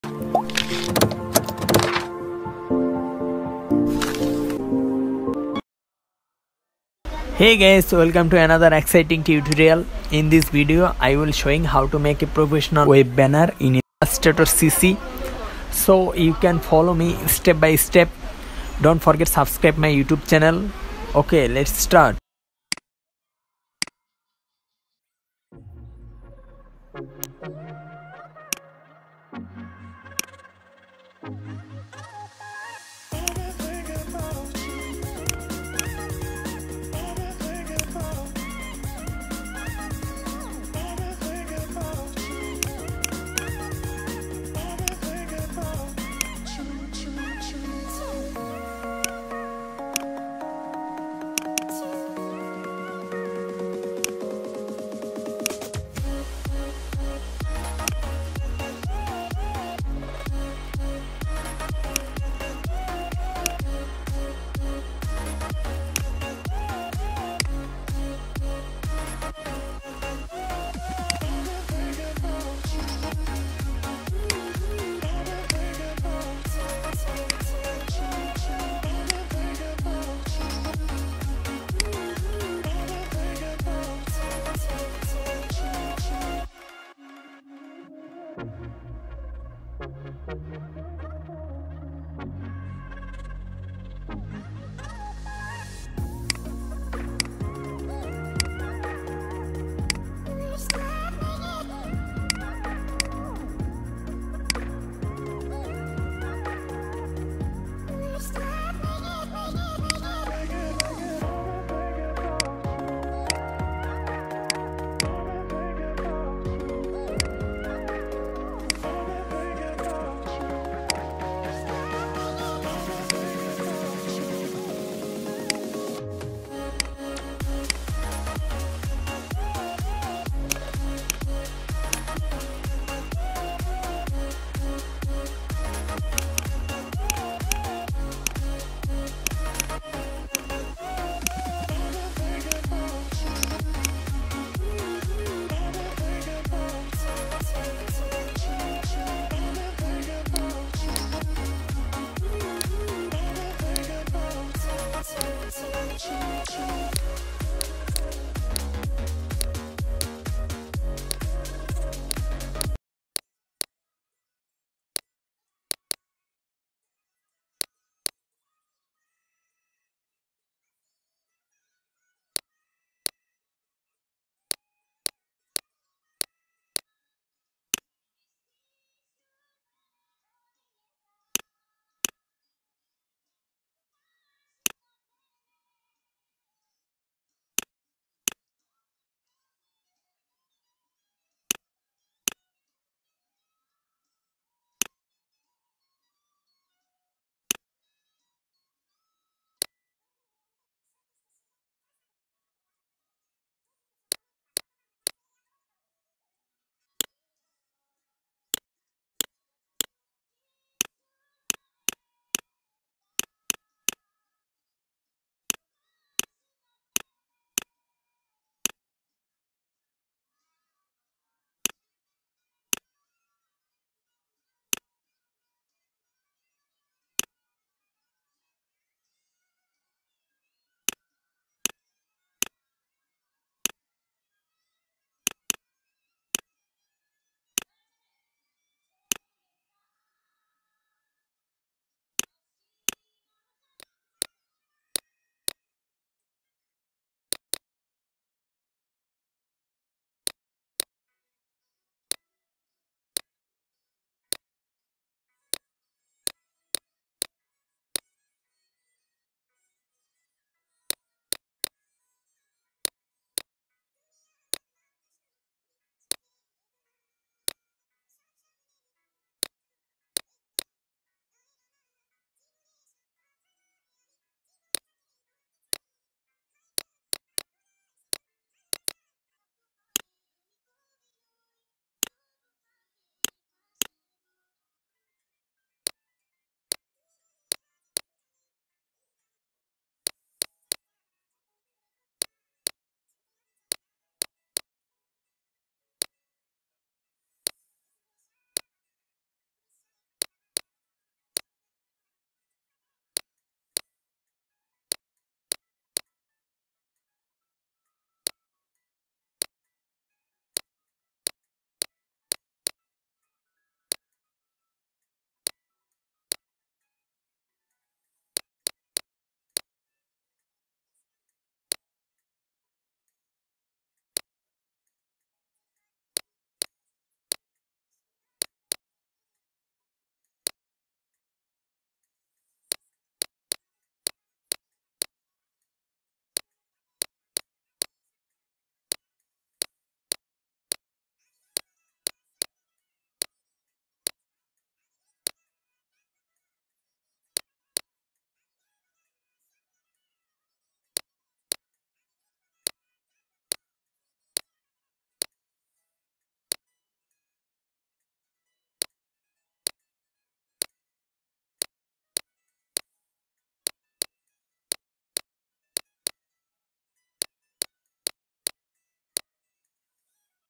how to make a professional web banner design in illustrator cc
how to make a professional web banner design in illustrator cc. .We will use Rectangle tool to create this banner in illustrator.  After watching this tutorial you Will be able to create you own designs such us Professional Web banner |

Hello Friend , I am Rohan Mahmud . Professional Web banner By Using Illustrator best of banner Tutorial-2020.This is Amazing Illustrator tutorial.This tutorial is about Web banner design.

Top Performing Banner Ad Sizes :
300×250 – Medium rectangle
336×280 – Large rectangle
728×90 – Leaderboard
300×600 – Half page or large skyscraper
320×50 – Mobile leaderboard

Most Common And Used Standard Banner Ad Sizes:
Square – 250 x 250
Small Square – 200 x 200
Banner – 468 x 60
Leaderboard – 728 x 90
Inline Rectangle – 300 x 250
Large Rectangle – 336 x 280
Skyscraper – 120 x 600
Wide Skyscraper – 160 x 600
Half-Page Ad – 300 x 600
Large Leaderboard – 970 x 9

photo source : unsplash